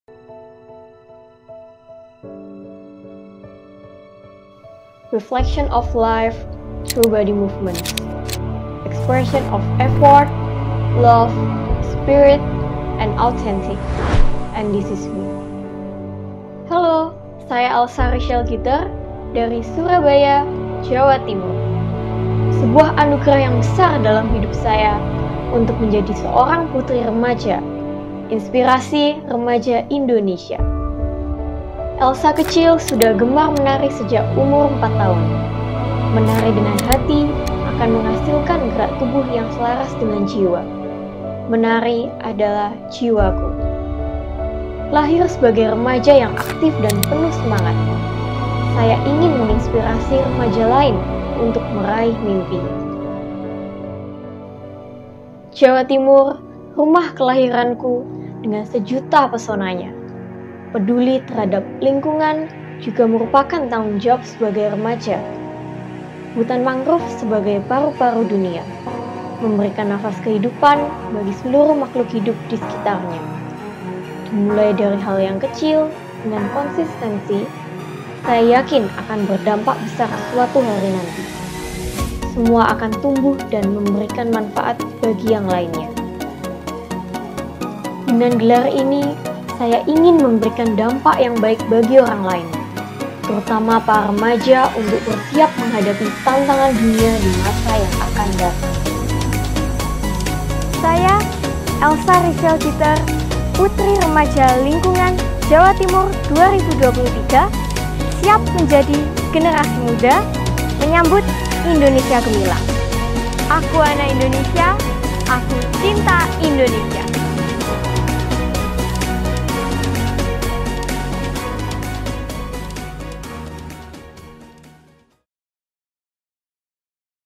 Profil puteri remaja Indonesia 2023 - Elsha Richelle (Jawa Timur 3)
Di video ini yayasan memperkenalkan finalis Puteri Remaja Indonesia 2023. Dengan mempresentasikan daerah asal finalis. Dengan video ini harapkan finalis dapat menjadi duta daerah nya dalam mempromosikan potensi wisata dan daerah asal finalis.

Dukung finalis favorite untuk mendapatkan atribut video presentasi terbaik, dengan cara like, komen dan share video ini sebanyak banyak nya.